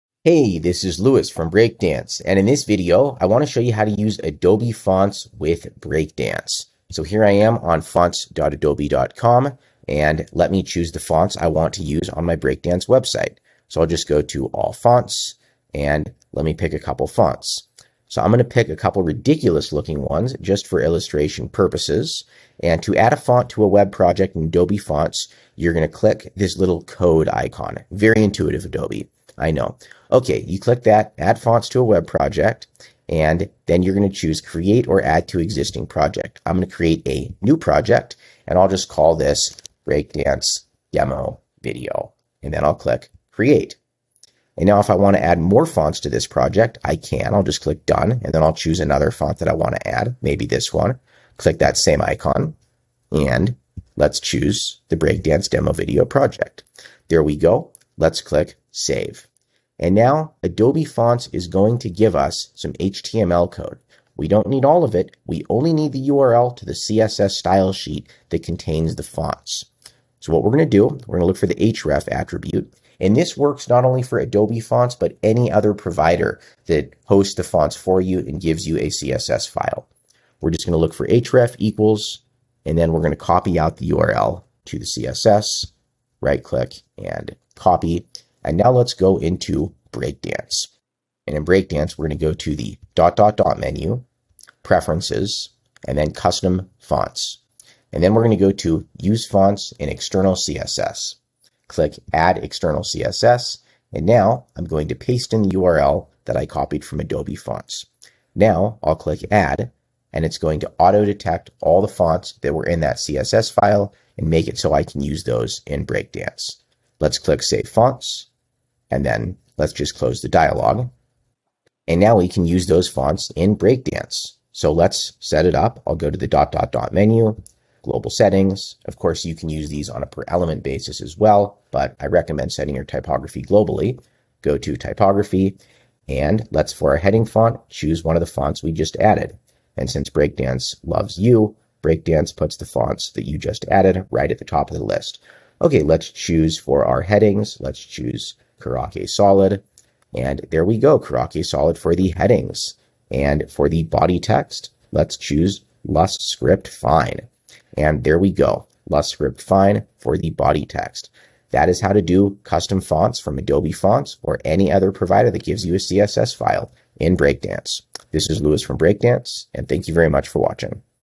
How To Use Adobe Fonts in Breakdance
These same steps will work with any 3rd party font provider that hosts the fonts and provides you with a CSS file.